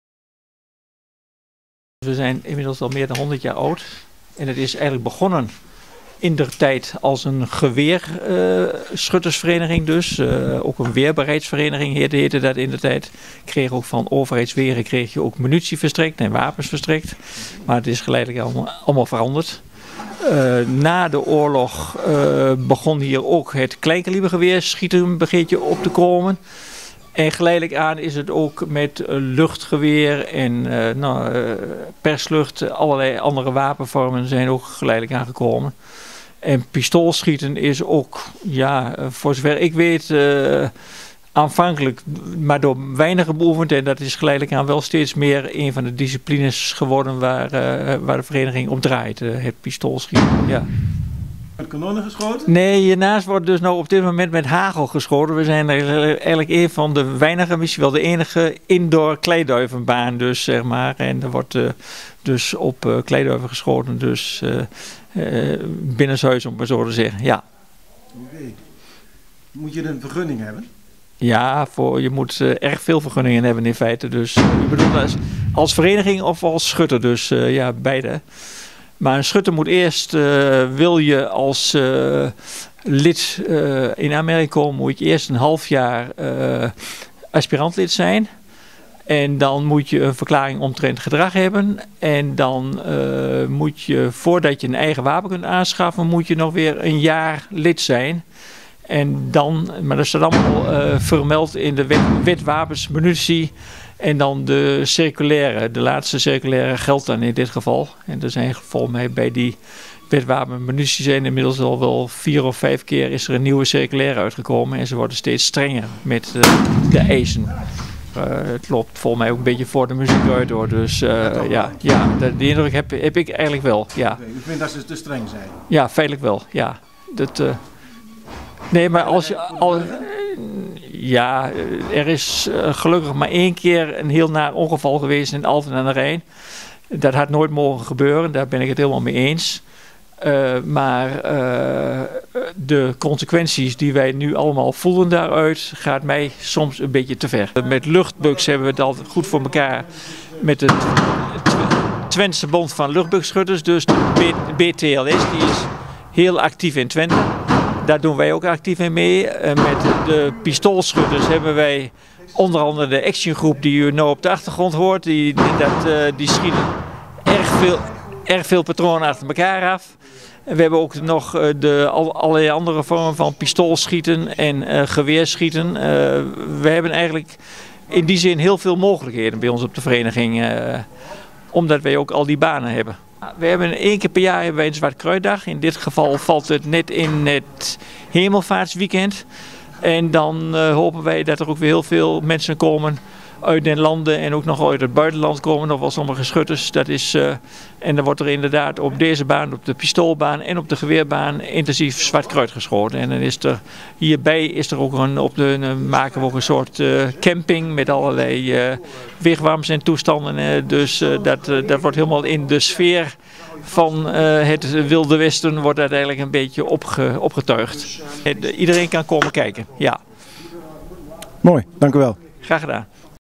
SDVV Almelo organiseert zwartkruit-dag begin mei
Een bijzondere vereniging in Almelo die al 105 jaar bestaat is de SDVV Almelo. De schietvereniging aan de Badweg houdt in het weekend na hemelvaart weer een "zwart kruitdag", een openhuis waar iedereen kennis kan maken met de schietsport.

Er is dan een heel programma waar gasten uit het hele land op af komen. Het wordt in de sfeer van "het Wilde Westen" opgetuigd. In de video verteld voorzitter Hans Brinkhorst over de vereniging.